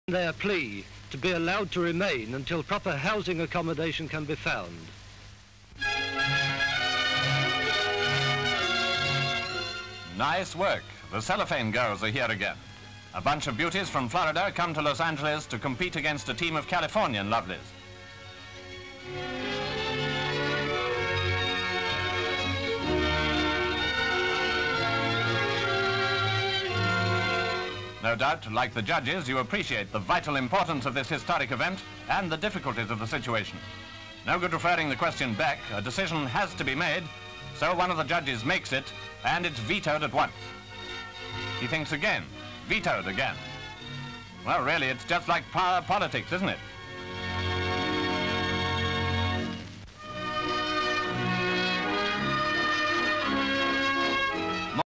POSTER GIRLS
(5 Aug 1946) A contest between the lovelies of Florida and California.